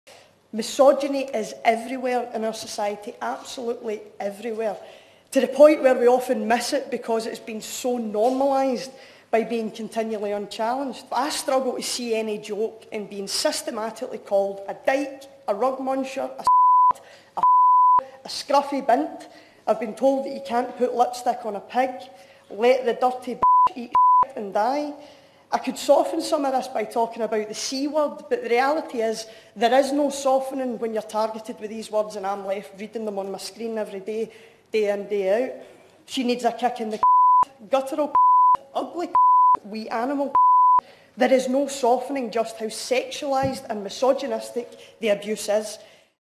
Mhairi Black reveals torrent of online misogynistic abuse - Daily Mail
The SNP's Mhairi Black revealed the scale of misogynistic abuse she faces online, including regularly being called the C-word, during a Westminster Hall debate in London where calls were made for misogyny to be treated as a hate crime.

Credit: PA